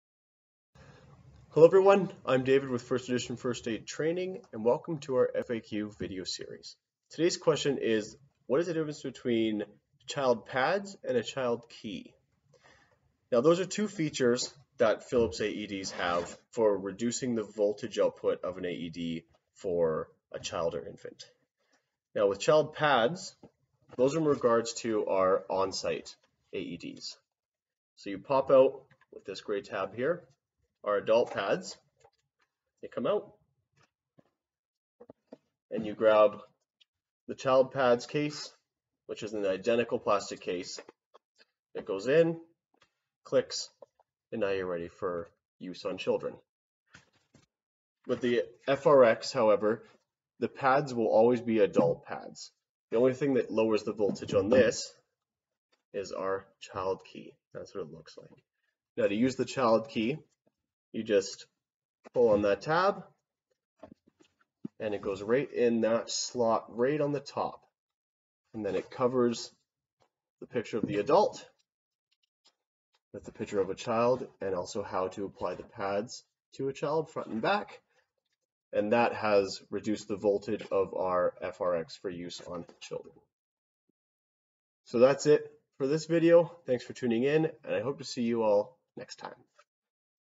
FAQ: What Is The Difference Between AED Child Pads And A Child Key?
Philips uses both of these features to reduce the voltage output of an AED for a child or infant, but what are the differences between them?

Today's video discusses AED child pads and child keys, what they do, and how to use them properly. We will show you exactly how to change out adult pads for child ones, as well as how to properly insert a child key.

HeartStart Infant/Child Smart Pads are designed to be used only on children or infants under 8 years of age or weighing less than 55 Ibs.

The unique Infant/Child Key is used only with the FRx Defibrillator when treating an infant or child who is under 55 lbs or 8 years old.